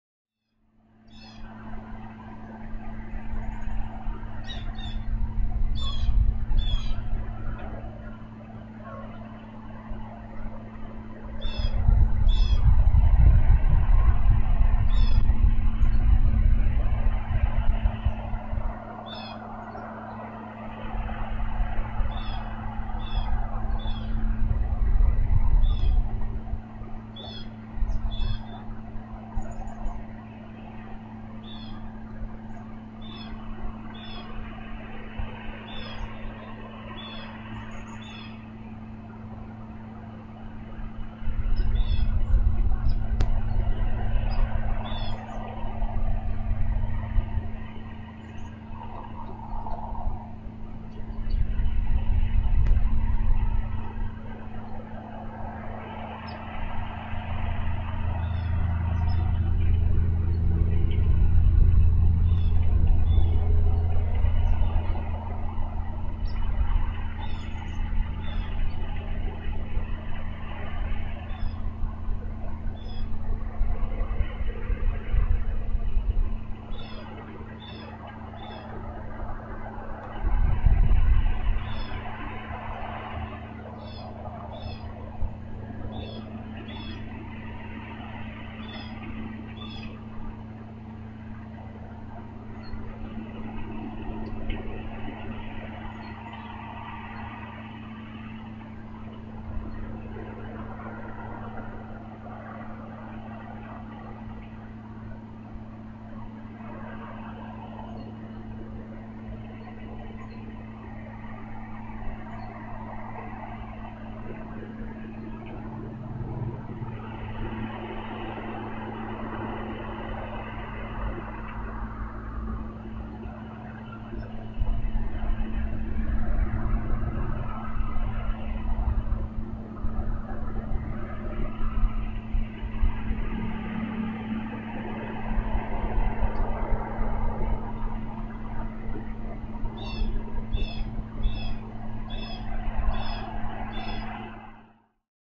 MNBound Eagles Dad's Tongue 3-27-13
Now that's a close up! Many thanks to the camera operators, Mark, Anthony & Lance. The eggs were laid on Mar 9th & 12th & the estimated hatch date is Apr 14th (give or take a day). Recorded at 11.17am CDT.